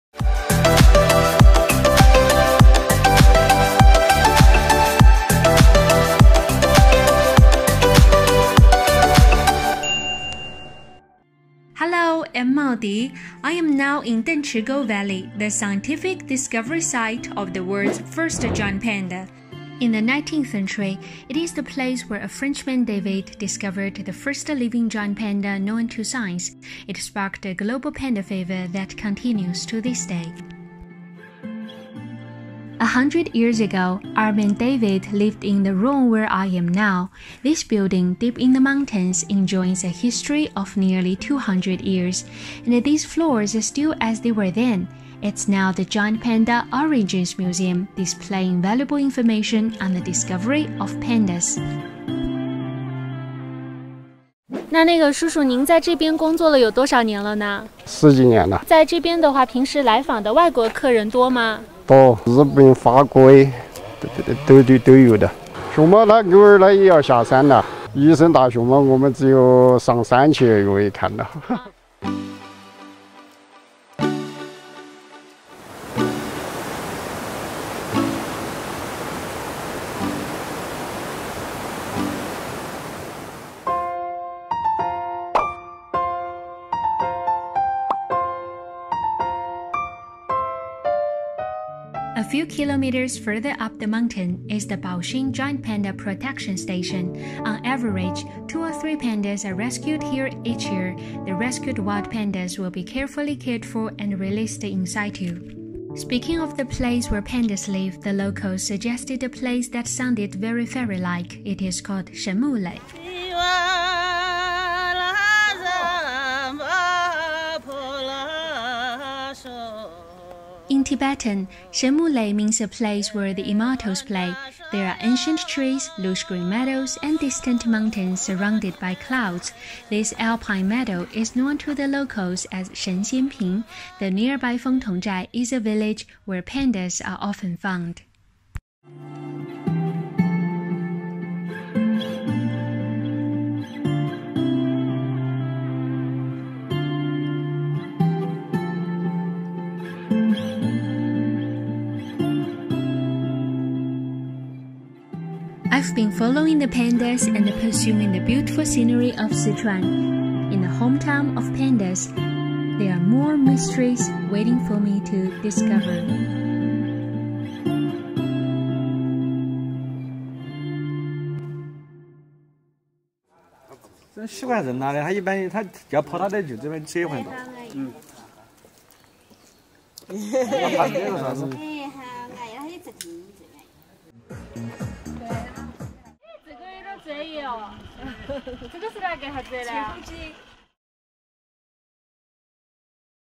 A visit to the scientific site of the first giant panda discovery
With elevations ranging from 188 meters to 7,556 meters, Sichuan boasts wonderful natural scenery and unique landscapes in one of the most biodiverse regions in the world.

Departing from Chengdu, I headed west to Dengchigou, the world’s first giant panda discovery site, via the Chengdu-Ya’an Expressway and S210 Highway. I visited the Dengchigou Giant Panda Museum, an ancient all-wooden structure. It was also the living quarters of the French biologist David, who first discovered the giant panda. Therefore, Baoxing county, where Dengchigou is located, is also known as the “Hometown of the Pandas.”

Along the route, I visited Fengtongzhai National Nature Reserve Museum, where I learned about the cultural history and the stories of the giant panda that had gone global. I also went to neighboring Shenmulei Scenic Area of Qiaoqi township, to admire the Tibetan folklore and the sound of heaven through the area's original multi-part folk songs.

This is the first episode in the vlog series titled "A Visit to the Hometown of Pandas" for the China (Sichuan) Giant Panda Culture and Tourism Week (Online) 2020. Follow us to see more.